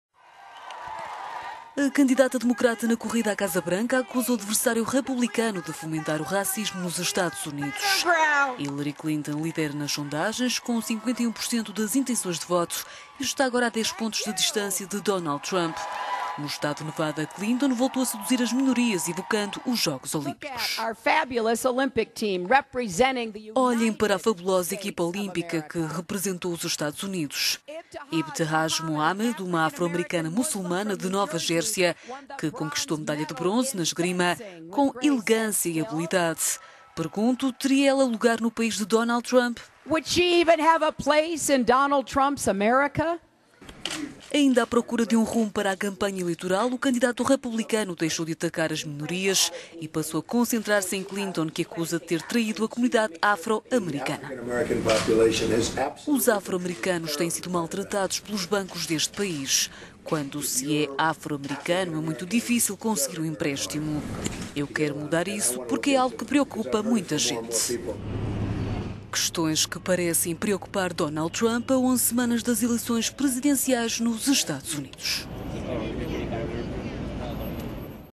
Clinton e Trump trocam acusações de olhos postos nas minorias
A candidata democrata na corrida à Casa Branca acusa o adversário republicano de fomentar o racismo nos Estados Unidos.

Hillary Clinton lidera nas sondagens com 51 por cento das intenções de voto e está, agora, a 10 pontos de distância de Donald Trump.

No Estado do Nevada, Clinton voltou a seduzir as minorias evocando os Jogos Olímpicos no Rio de Janeiro.

"Olhem para a fabulosa equipa olímpica que representou os Estados Unidos, Ibtihaj Muhammad, uma afro-americana muçulmana de Nova Jérsia …